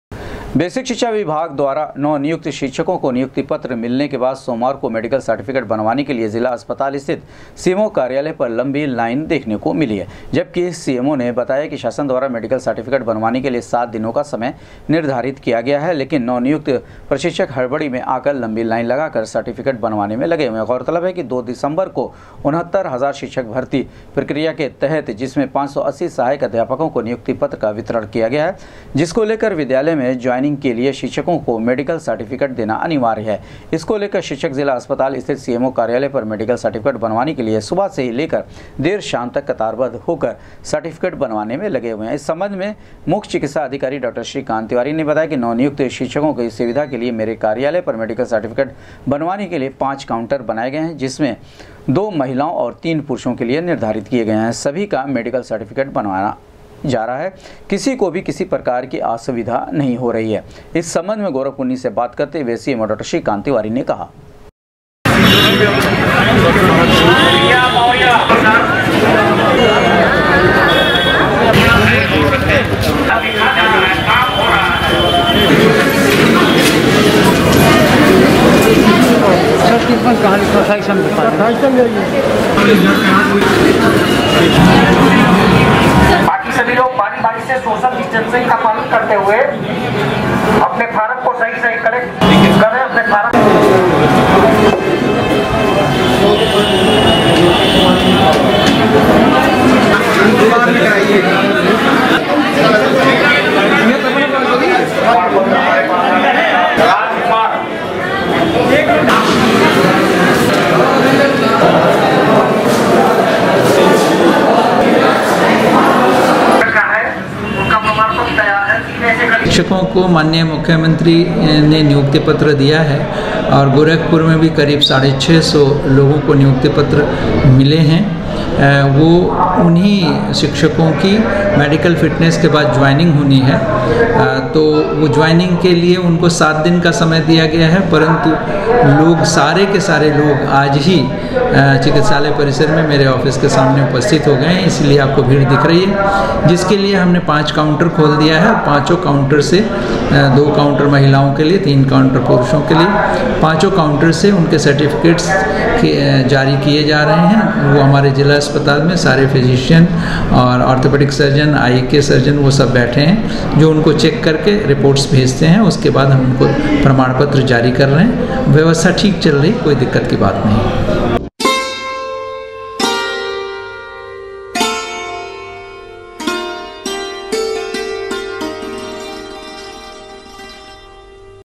सीएमओ कार्यालय पर मेडिकल सर्टिफिकेट बनवाने के लिए उमड़ा नवनियुक्त शिक्षकों का हुजूम, जबकि सीएमओ ने
सीएमओ कार्यालय पर मेडिकल सर्टिफिकेट बनवाने के लिए उमड़ा नवनियुक्त शिक्षकों का हुजूम, जबकि सीएमओ ने कहा, मेडिकल सर्टिफिकेट बनवाने के लिए 7 दिनों की है मोहलत